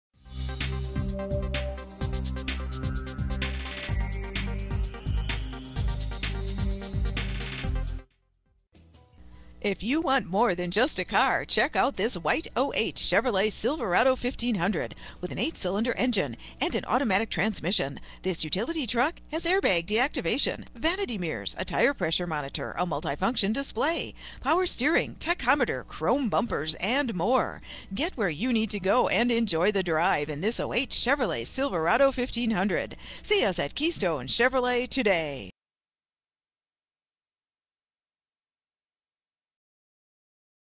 2008 Chevrolet Silverado 1500 Keystone Chevrolet Sand
2008 Chevrolet Silverado 1500- Keystone Chevrolet knows you want more than just a car. you have a purpose for your vehicle. check out this WHITE 2008 Chevrolet Silverado 1500, equipped with an 8 Cyl. engine and an automatic transmission. enjoy this utility truck with features like Airbag Deactivation, Vanity Mirrors, Tire Pressure Monitor, Multifunction Display, Power Steering, Tachometer, Chrome Bumpers, and much more. Get where you need to go, enjoy the drive, and have peace of mind in this 2008 Chevrolet Silverado 1500. see us at Keystone Chevrolet today!
vin- 1GCEC19C78Z143552

Keystone Chevrolet
8700 Charles Page Blvd.